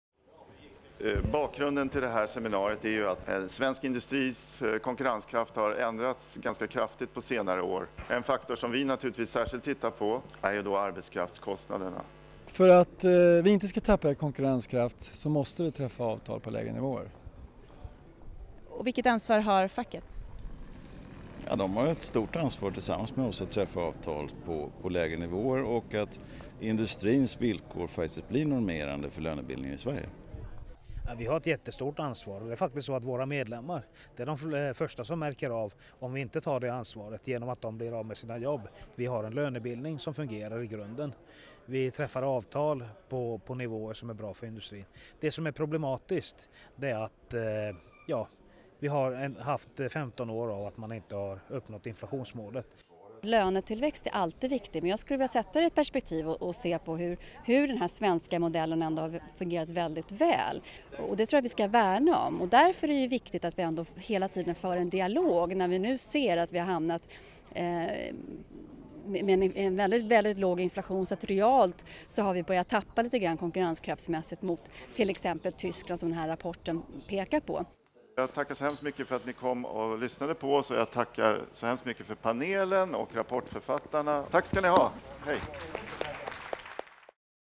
Osund lönebildning hämmar konkurrenskraften
Exportindustri tappar i konkurrenskraft genom höga reallöneökningar, en stark krona och en obefintlig inflation. Per Hidesten, VD, Industriarbetsgivarna, Anders Weihe, Förhandlingschef, Teknikföretagen, Veli-Pekka Säikkälä, Avtalssekreterare, IF Metall och Marie Giertz, chefsekonom, SEK. kommenterar lönebildningen och svensk konkurrenskraft.